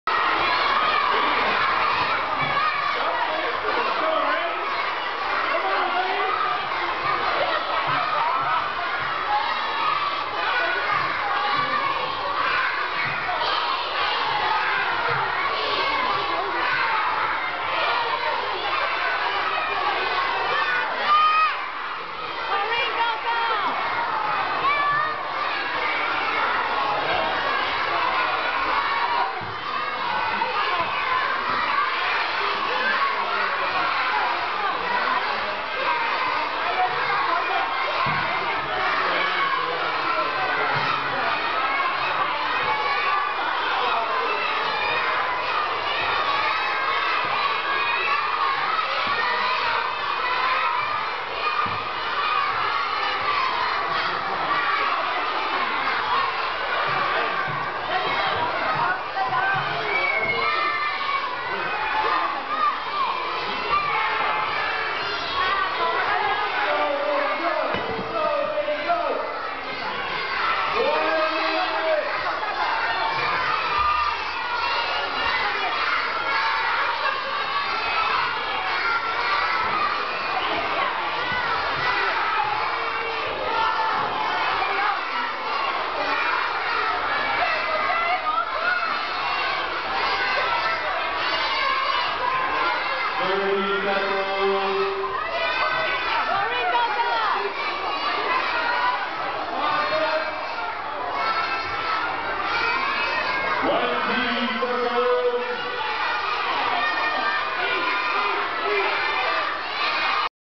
Frank's 2011 watermelon eating contest (student edition)
Alex versus Charlene/Jeff and more others not in the video.